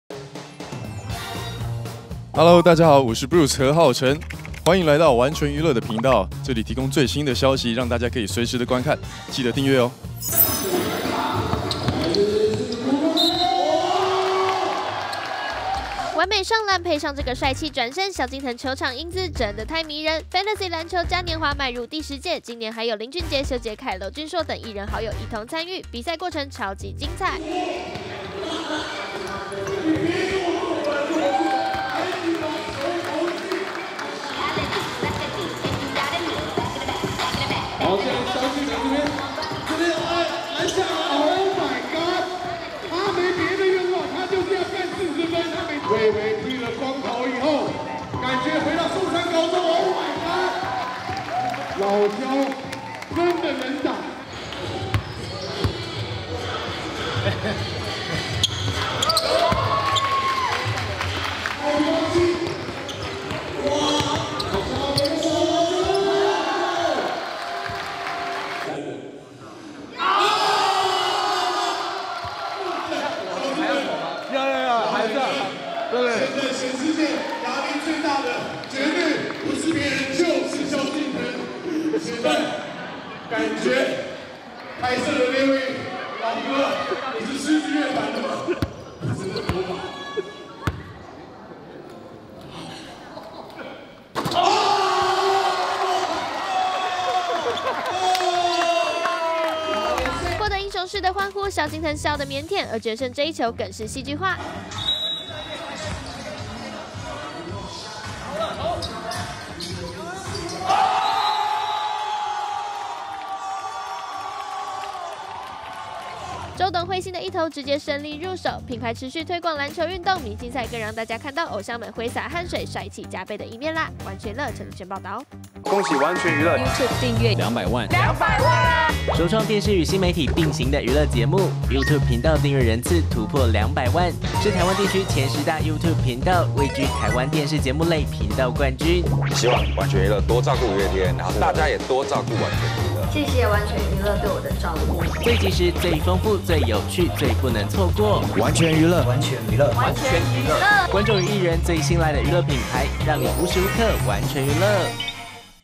【跪著看】周杰倫.林俊傑.蕭敬騰都來了 超大咖球賽天王們神凱瑞投籃帥瘋了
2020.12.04
完全娛樂